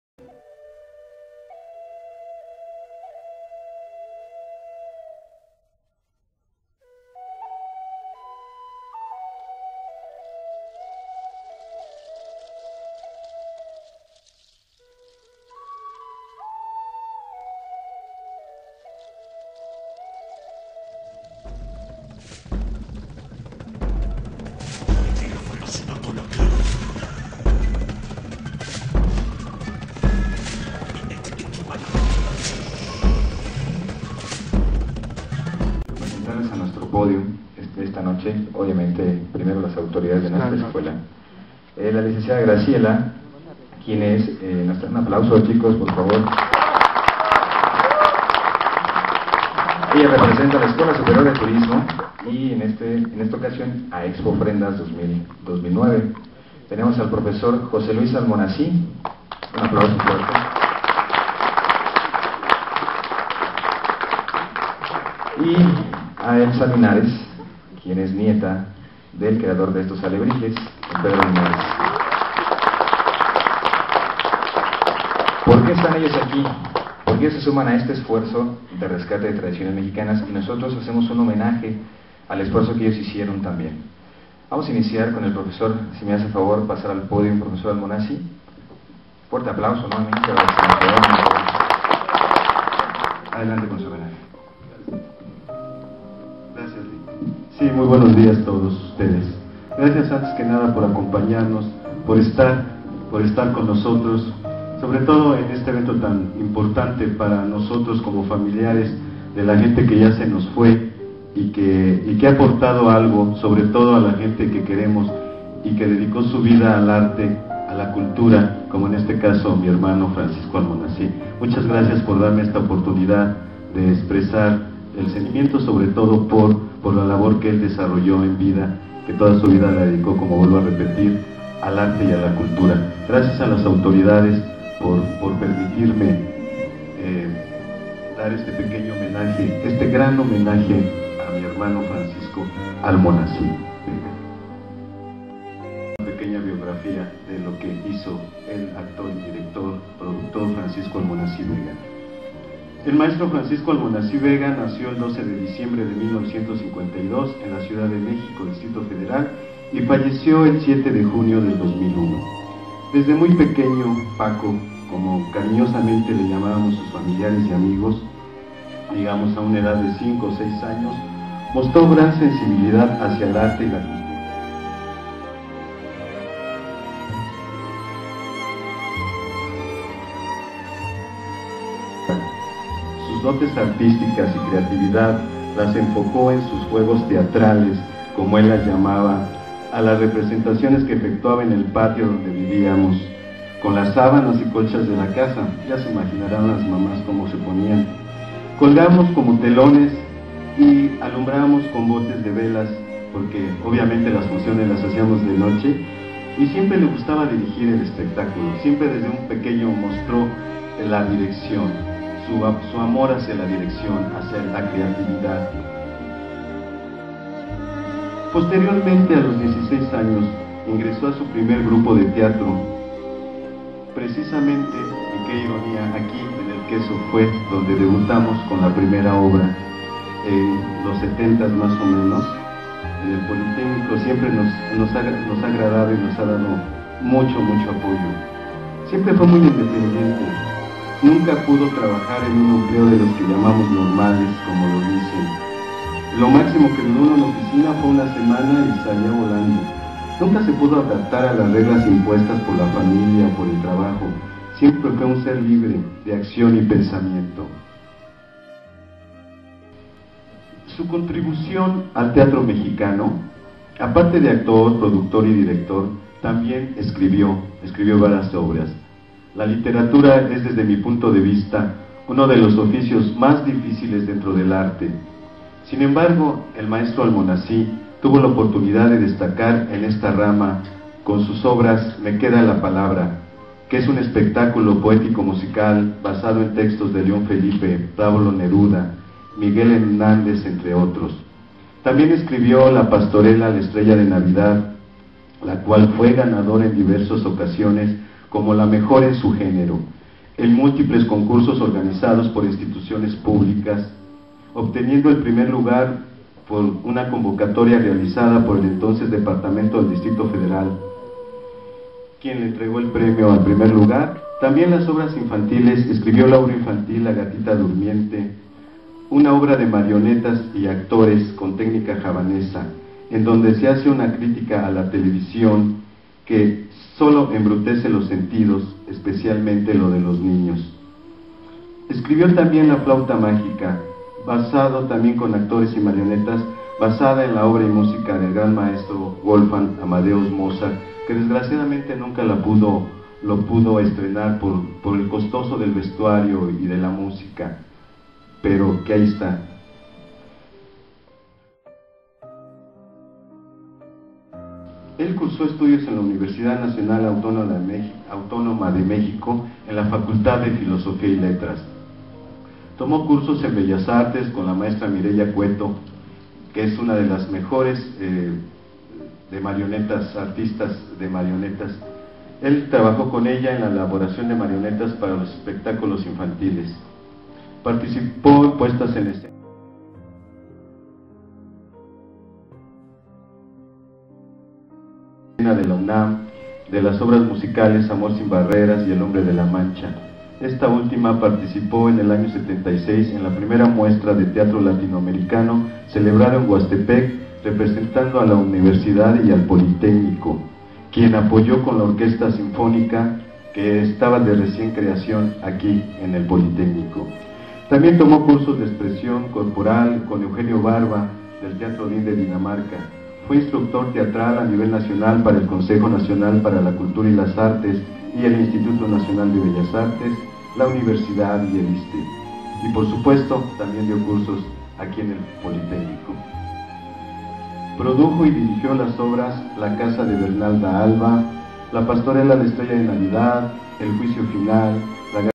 OFRENDAS 2009 EST - IPN "HOMENAJE A FRANCISCO ALMONACÍ 1a PARTE " RUTAS MAGICAS
MERECIDISIMO HOMENAJE A ESTE HOMBRE QUE CONTRIBUYO COMO NOSOTROS A ESTE RESCATE DE TRADICIONES MEXICANAS EN EL ARTE.